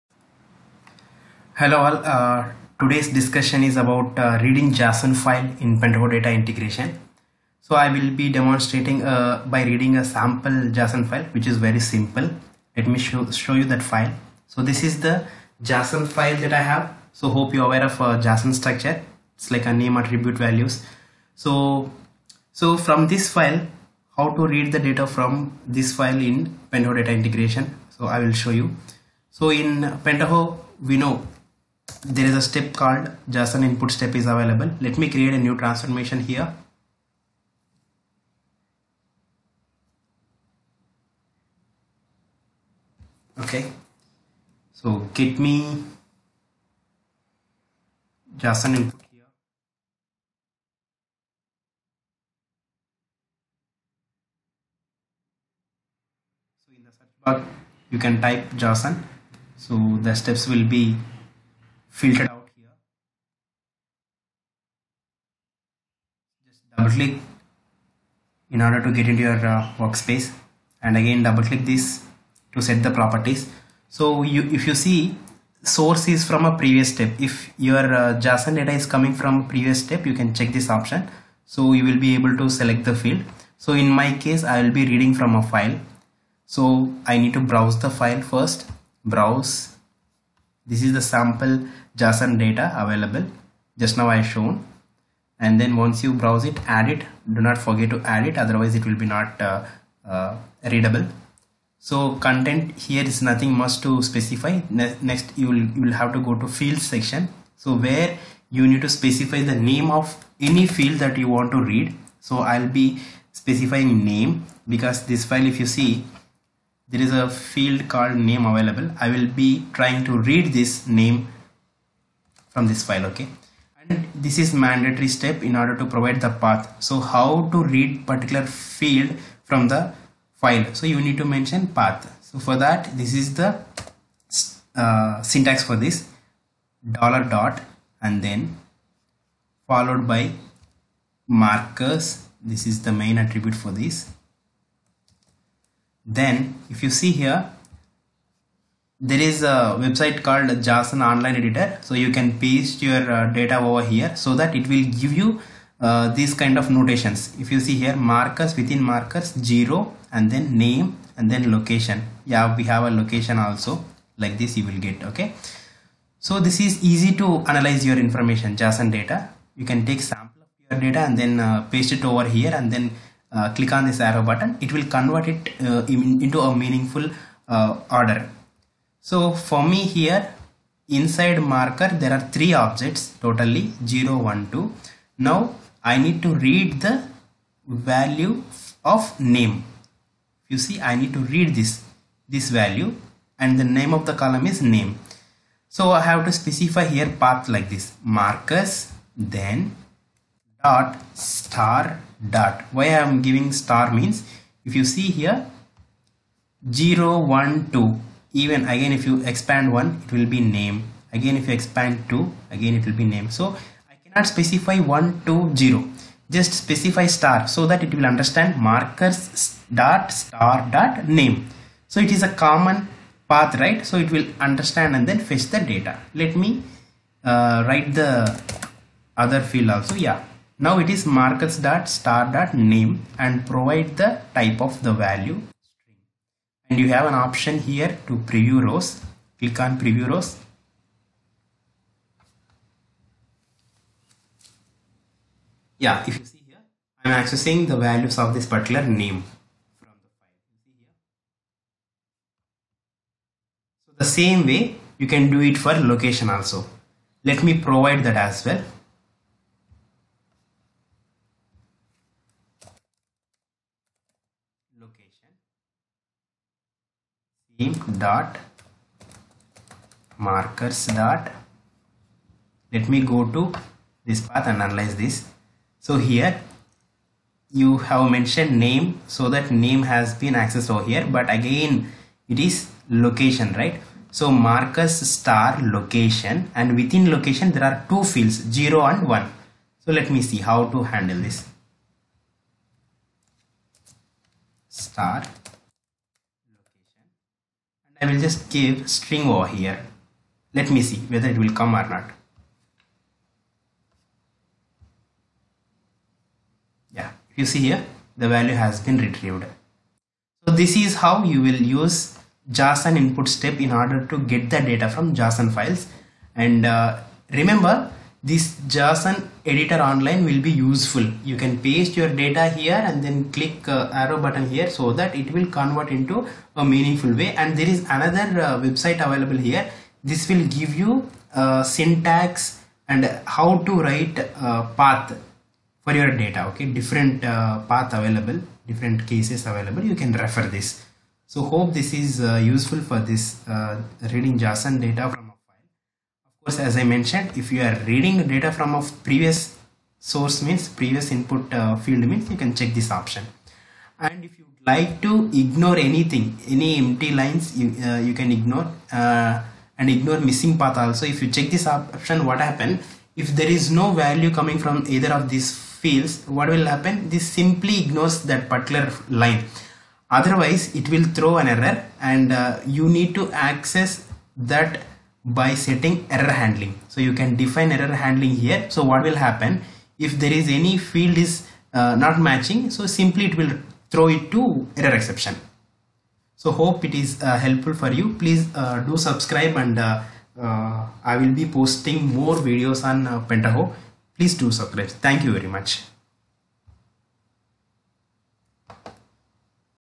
processing/reading json data in Pentaho Data integration
This video explains how to process json data using PDI.